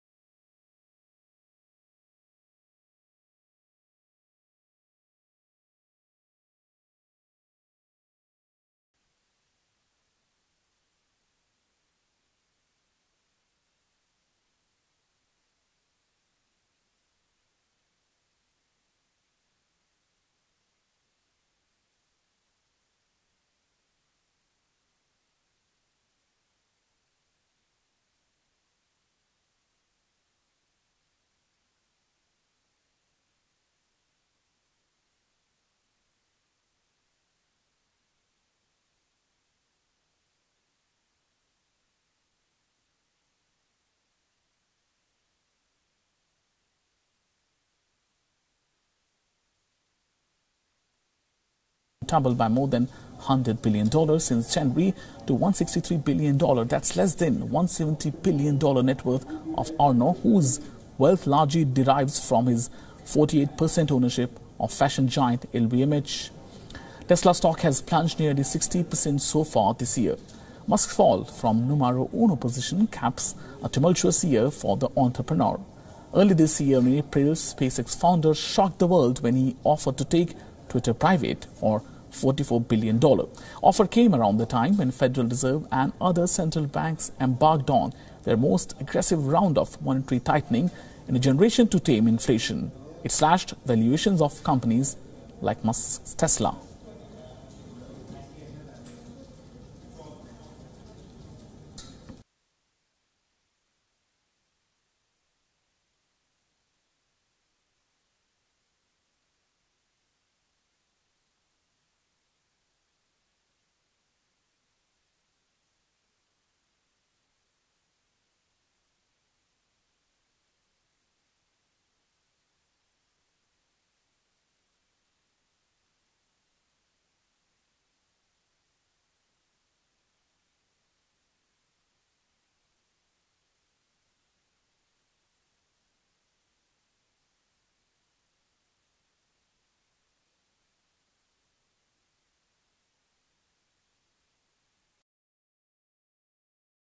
World Business Watch Elon Musk no longer world's richest, loses tile to LVMH's Arnault
Elon Musk is no longer richest man in the world, LVMH's Arnault has taken over Musk as world's richest person. Tesla CEO's wealth has been cut in half from 340 billion dollar peak. The major drop in wealth was due to Twitter purchase